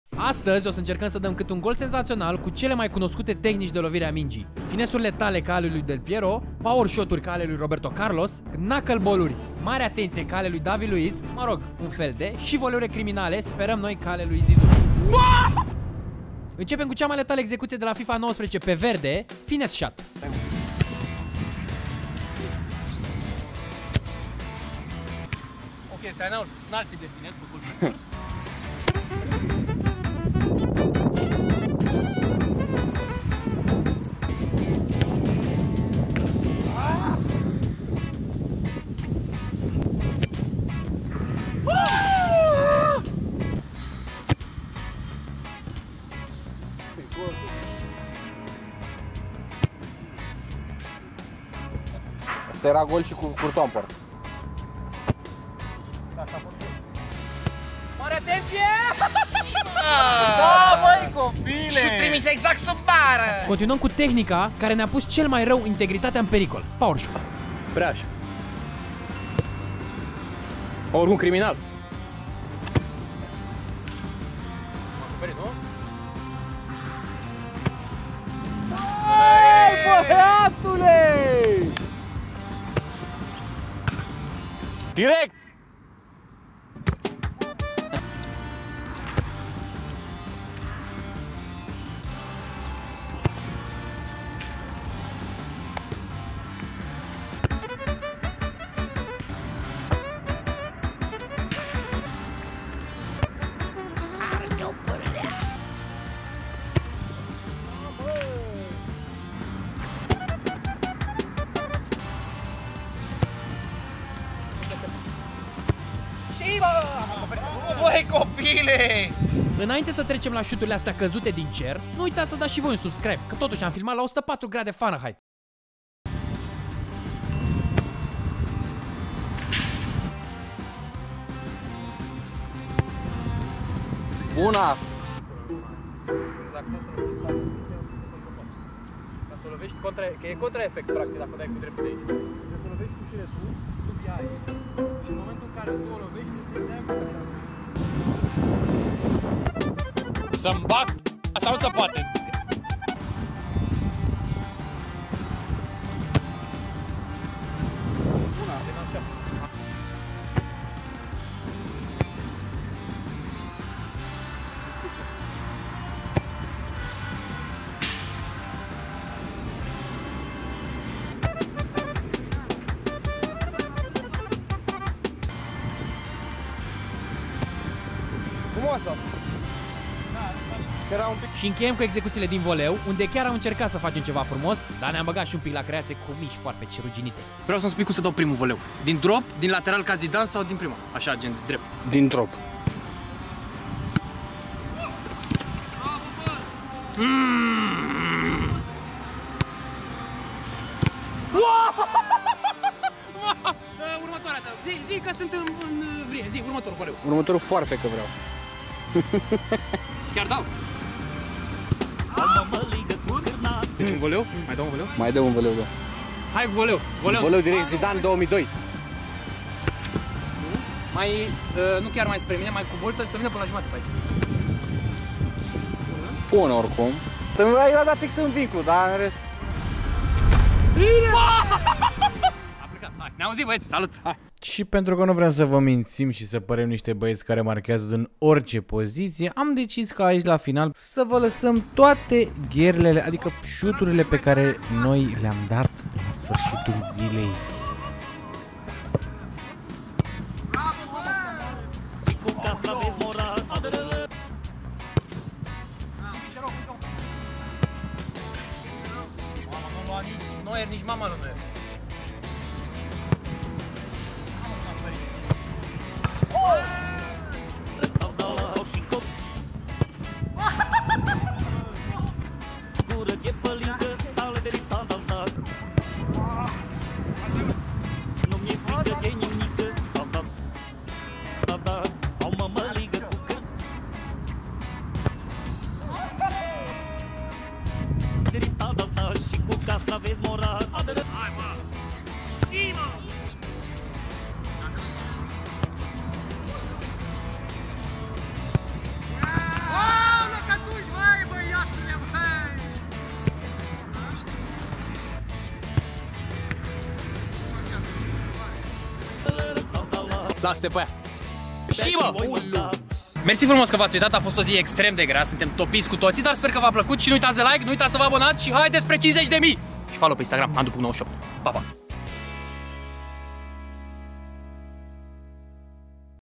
Inscriu UN GOL SENZATIONAL cu fiecare TEHNICA
În clipul de astăzi am încercat alături de prietenii mei: Liviu, Tibi și Dani să înscriem un gol de mare spectacol și calibru cu cele mai populare tehnici de șut din fotbal: Finesse, Power Shot, Knuckleball

S-au dat și mari gherle dar și câteva goluri drăguțe, așa că vă dorim vizionare plăcută

Drumul spre 50.000 A ÎNCEPUT ACUM

Andu Fotbal

Andu goluri

Goluri senzaționale